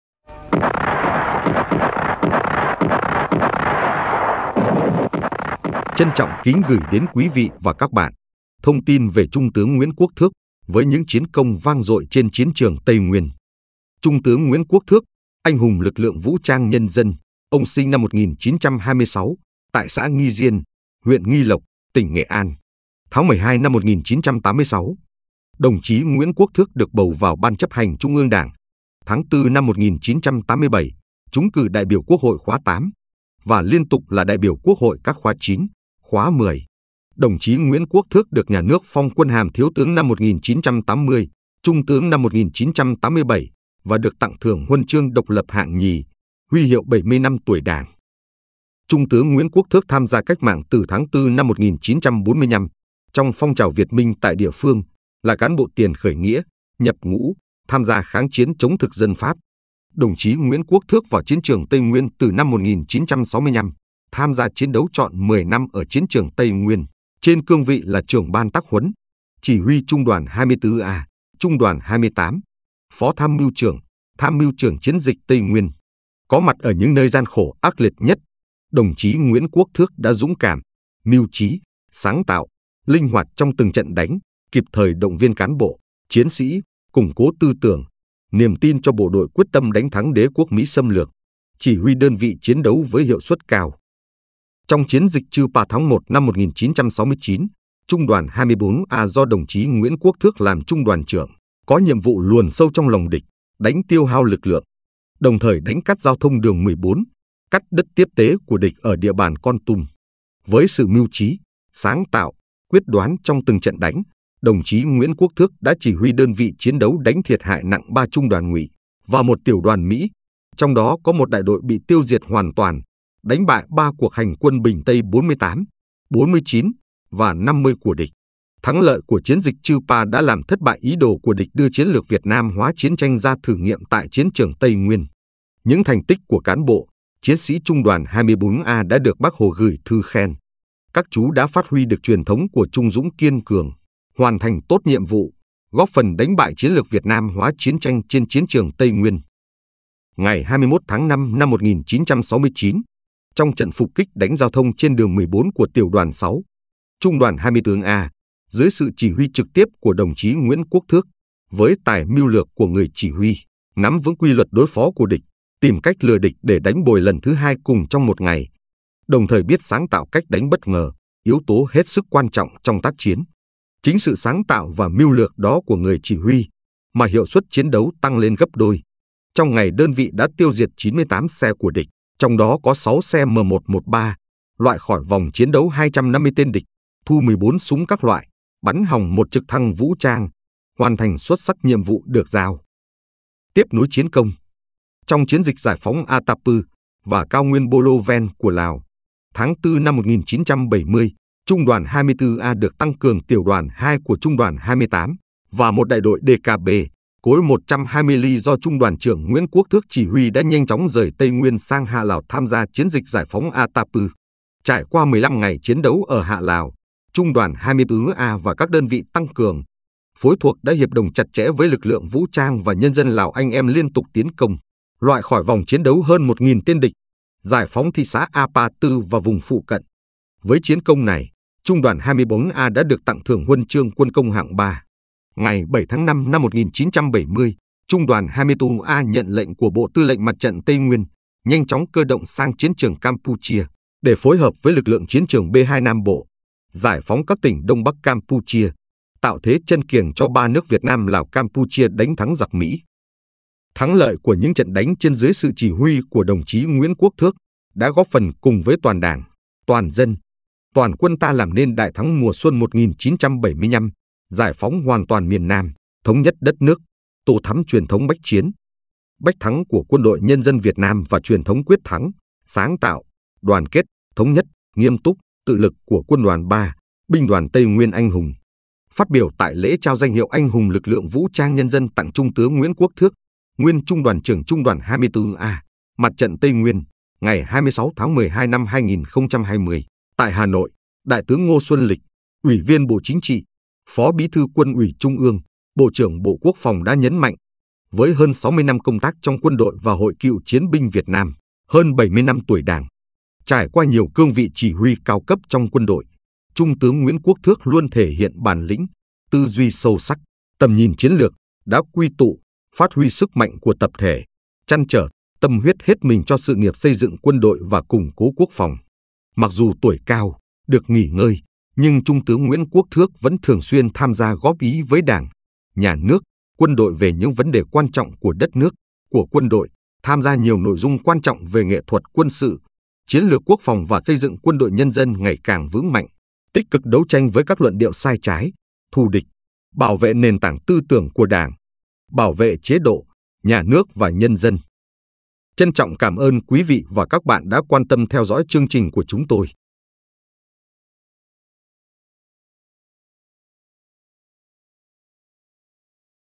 TRUNG TƯỚNG NGUYỄN QUỐC THƯỚC 10 NĂM TRÊN CHIẾN TRƯỜNG TÂY NGUYÊN.
Trân trọng kính gửi đến quý vị và các bạn những thông tin về đóng góp của trung tướng,anh hùng lực lượng vũ trang nhân dân nguyễn quốc thước trên chiến trường tây nguyên.